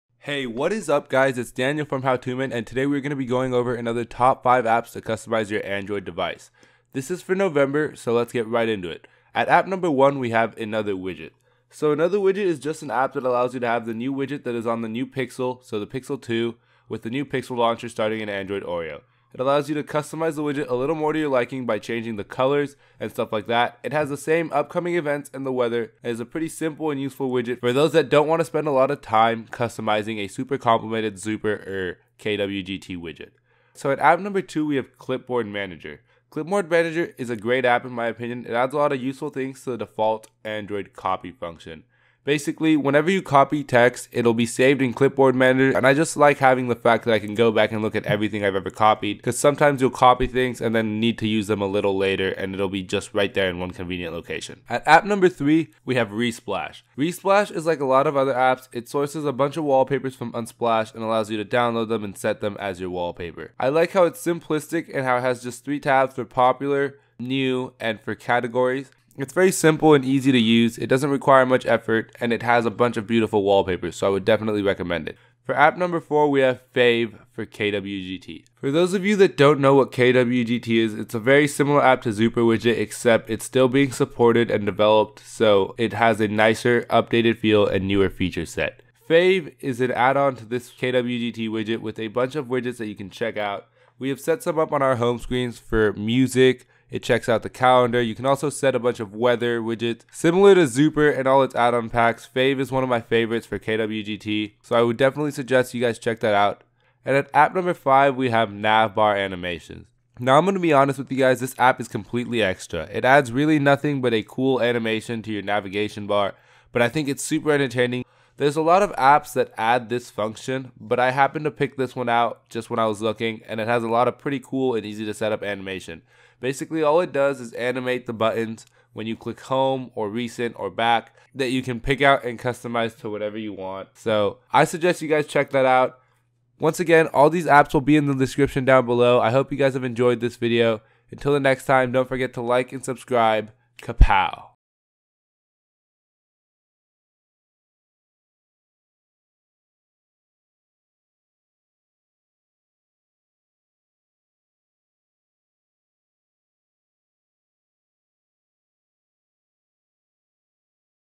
Top 5 Apps to Customize your Android Device November 2017!
1. Another Widget

Another widget is an app that allows you to have the new widget that is on the pixel launcher starting in android oreo. It allows you to customize the widget a little more to your liking as well as keep the upcoming events and the weather. its a very simple and useful widget for those that dont want to spend time customizing a super complicated zooper widget.

2. Clipboard Manager

Clipboard manager is a great app that in my opinion adds a lot of useful things to the default android copy function. This app allows u to go back and look into the text you have copied as well manage it and do a lot of different actions such as edit and merge amongst others.
3. Resplash

Resplash is this weeks wallpaper app and its a very simplistic app that like a lot of other apps sources its images from unsplash. I like how simplisitc it is and how it just has three tabs for new popular and for

4. FAV for KWGT

FAV for KWGT is a very clean and large kwgt widget set that has a lot of options. for those that dont know KWGT is an app very similar to zooper widget but that is still being supported and with a nicer more updated feel and feature set. With Fav we set up some very nice homescreen widgets so we suggest you check them out/

5.

Nav bar animations is a completely extra app but i happen to think its really entertaining. There are a lot of apps that have this function and so this is just one i found that is pretty easy to set up and has a lot of cool animations. Basically all it does is animate the nav bar when you click one of the buttons.

(⊙.⊙(☉_☉)⊙.⊙) ▏ Social ▏(⊙.⊙(☉_☉)⊙.⊙)